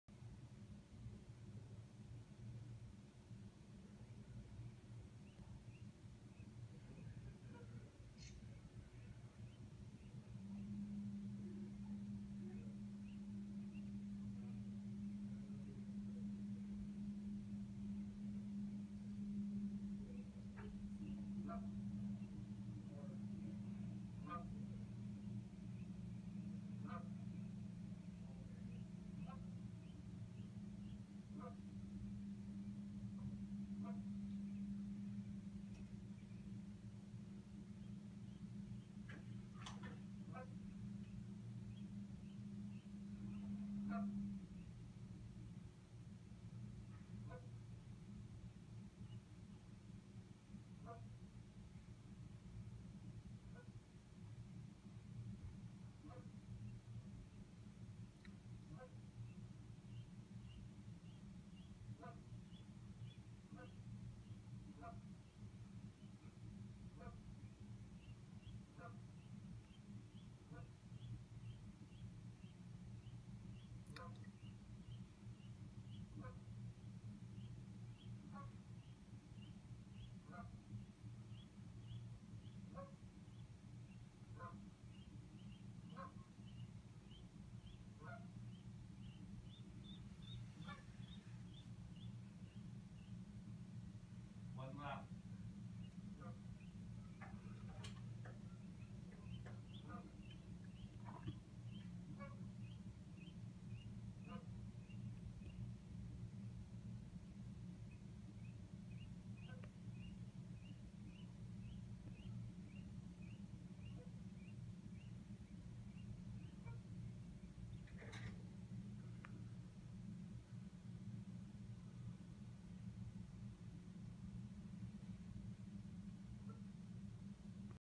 Taking the Leap at TMI!
Every year a family of Canadian Geese make the courtyard at our office their home. It's always an exciting day when the goslings take their first leap into the new and exciting world!